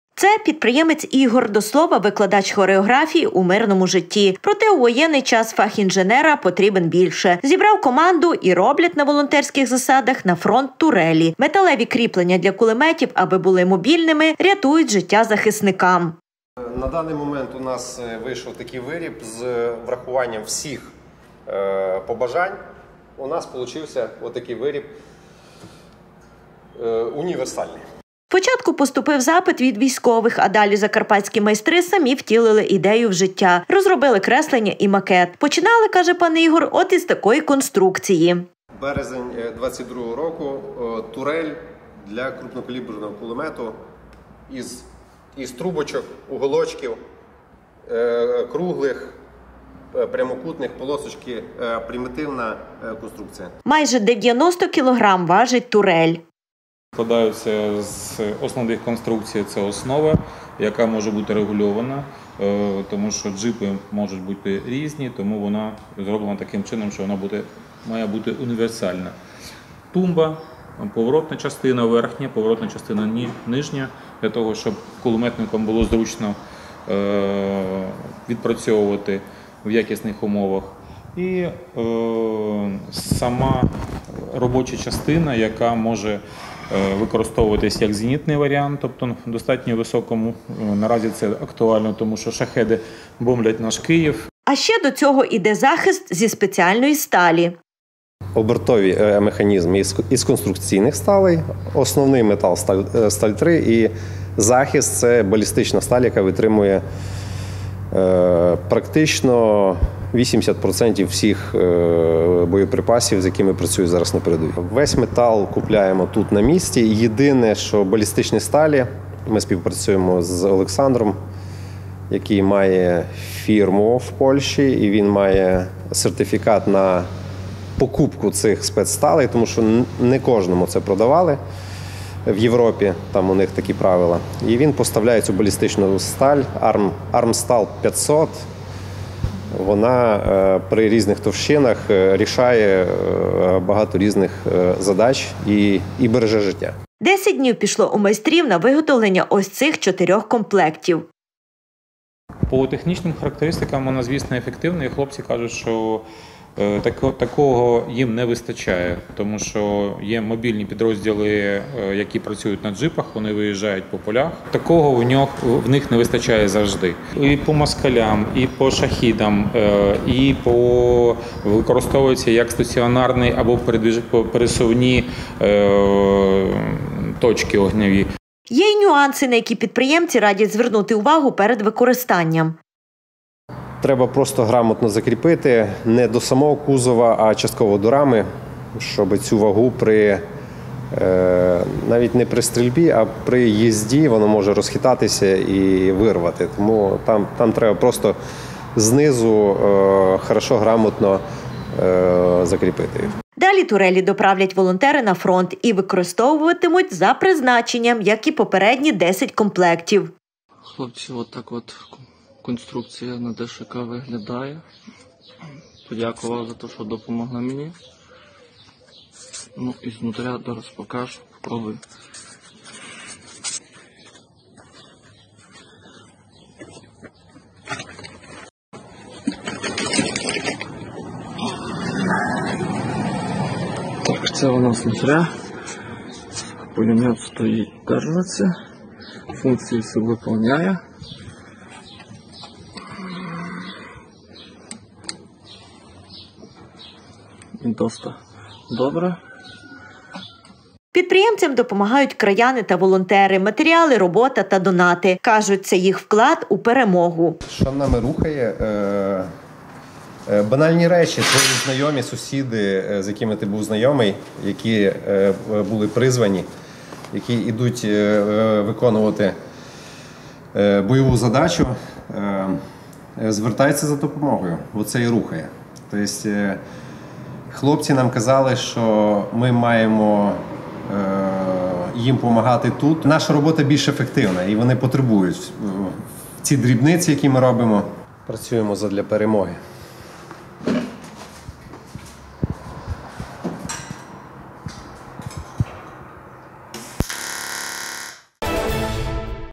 Турелі на фронт!
Закарпатські підприємці всіляко підтримують військових! Серед них -інженер Ігор та його команда. Спочатку відправляли на передову кішки для розмінування, потім бронежилети, автівки, зараз роблять триноги та  турелі, а також пічки для обігріву та приготування їжі. Словом, усе що потребують на фронті! Як у звичайному ангарі виготовляють кріплення для кулеметів. Це – підприємець Ігор, до слова, викладач хореографії – у мирному житті, проте у  воєнний час фах інженера - потрібен більше! Зібрав команду і роблять на волонтерських засадах на фронт турелі! Металеві кріплення для кулеметів, аби були мобільними, рятують життя захисникам!Спочатку поступив запит від військових, а далі закарпатські майстри самі втілили ідею у життя - розробили креслення і макет. Починали, каже пан Ігор, от із такої конструкції.  Майже 90 кг – важить турель.  А ще до нього іде захист зі спеціальної сталі.    10 днів пішло у майстрів на виготовлення ось цих 4 комплектів.  Є й нюанси, на які підприємці радять звернути увагу перед використанням!   Далі турелі доправлять волонтери на фронт! Кажуть, це їх вклад у Перемогу!